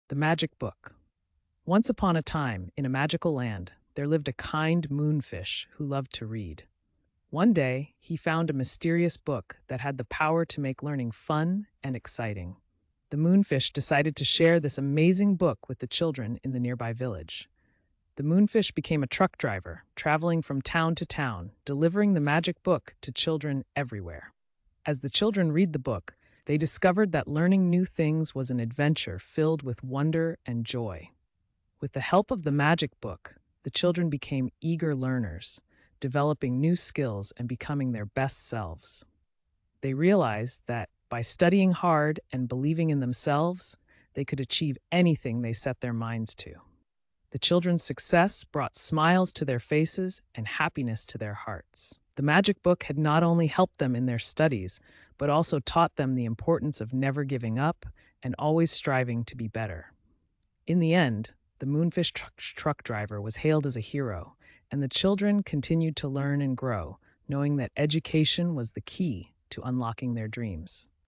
The Magic Book
Join the Moonfish truck driver on a magical journey to help children discover the joy of learning and self-development. Let the power of the magic book inspire you to reach for the stars! story fairytale tale # story tale MagicBook LearningIsFun BelieveInYourself Picture